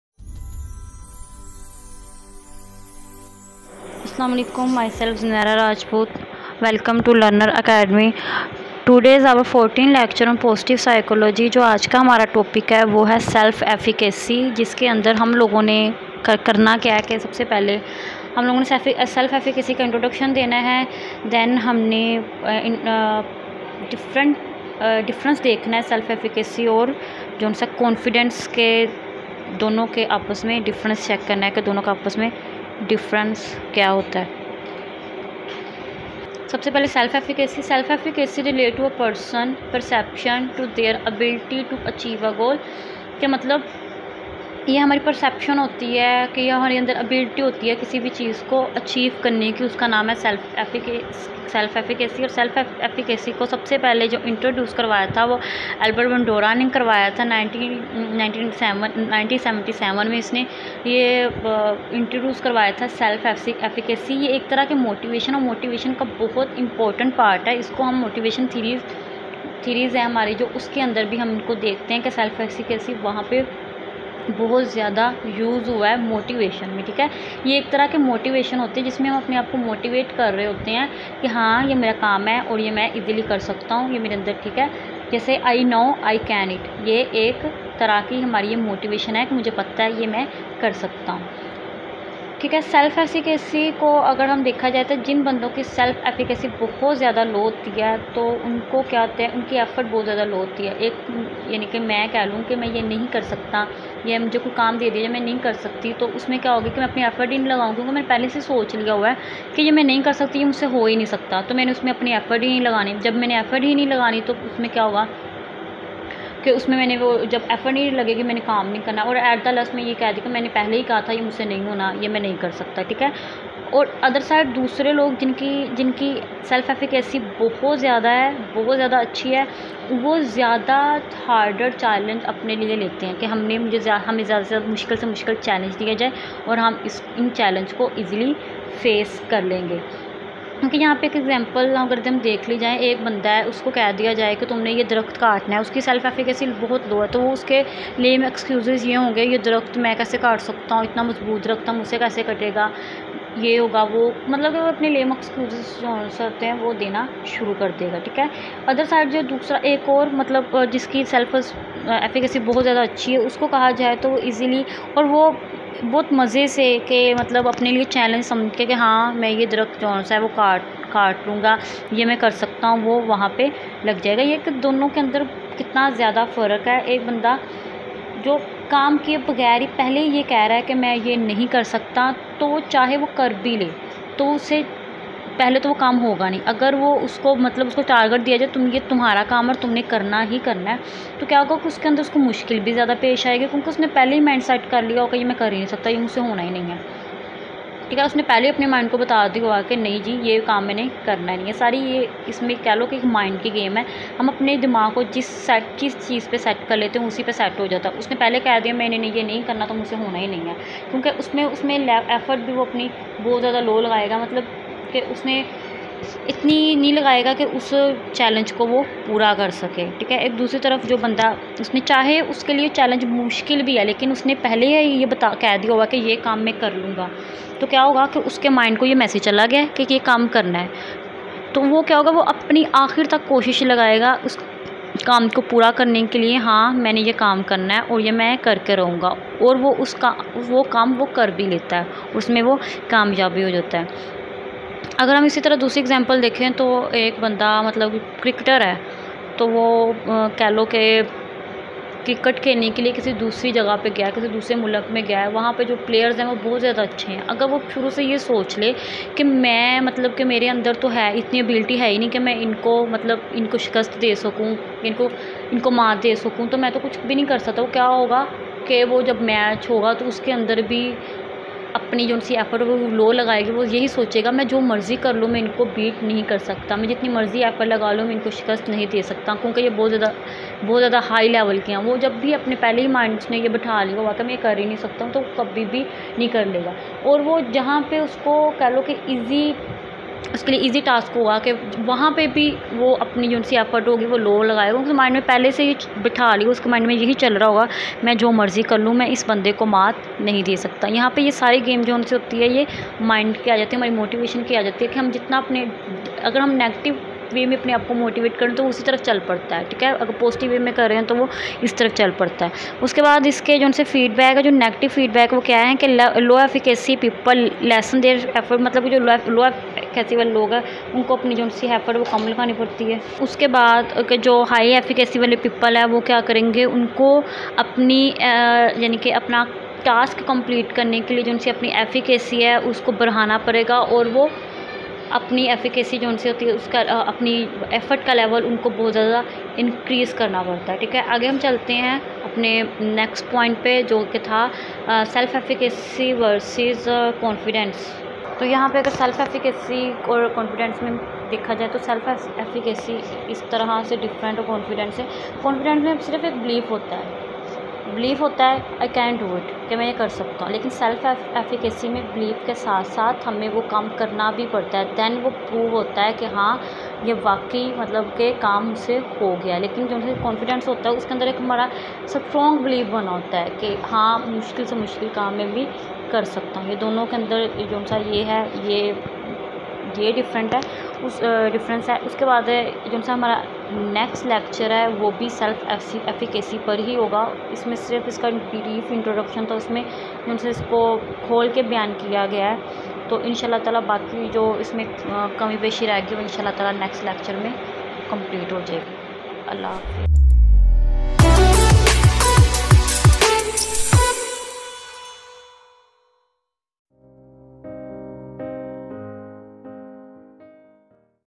Positive Psychology (PSY409)VU Urdu | Lecture No. 14| Learners Academy By Bia Baloch
♦•🛡️Learners Academy (Online) By Bia Baloch°⫸•♦

➡️Positive Psychology (PSY409)VU Urdu
➡️Self-Efficacy
By: Miss. Zunaira Rajpoot (Clinical Psychologist)